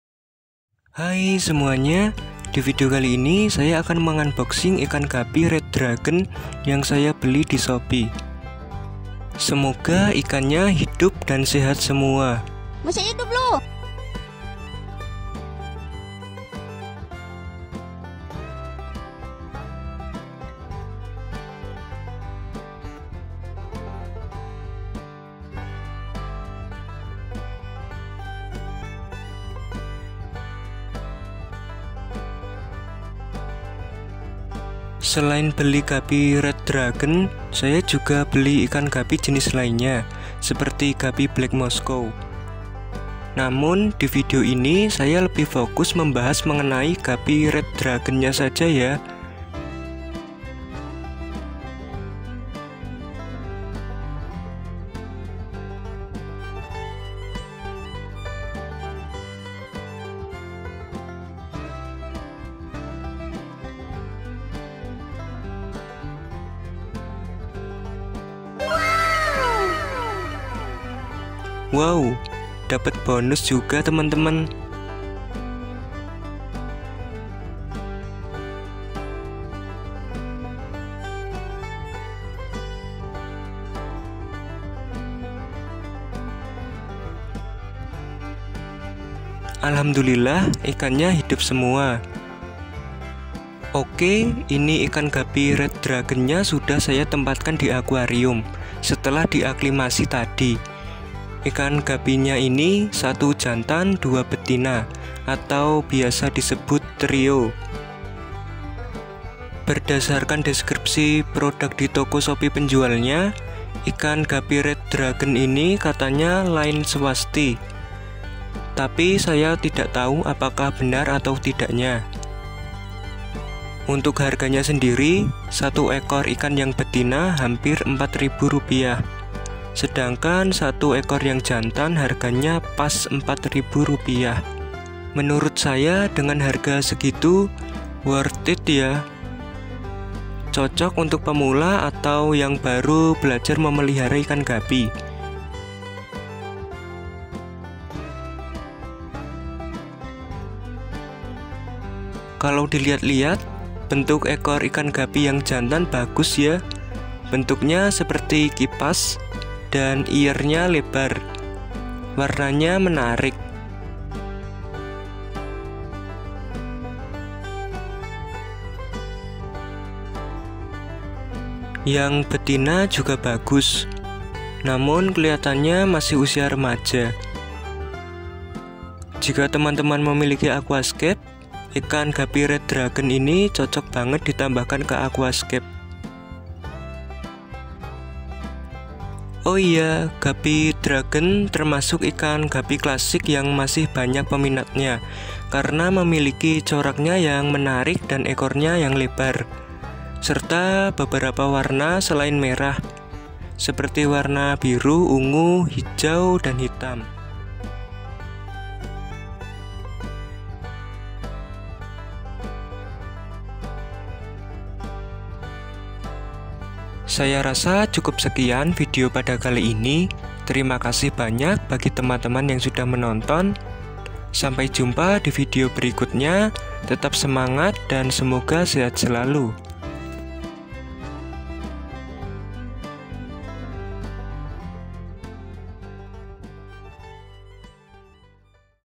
Unboxing Ikan Guppy Red Dragon Line Swasti dari Shopee | Cuma 4 Ribu Rupiah!!!
Hai semuanya :)

Di video kali ini saya akan meng - unboxing ikan guppy red dragon yang saya beli di shopee. Selain membeli ikan guppy red dragon, saya juga beli ikan guppy jenis lainnya. Seperti guppy black moscow, guppy afr (albino full red), dan guppy japan blue red mozaik. Namun, di video ini saya lebih fokus membahas mengenai guppy red dragonnya saja ya.

Terima kasih banyak bagi teman-teman yang sudah menonton. Sampai jumpa di video berikutnya. Tetap semangat dan semoga sehat selalu.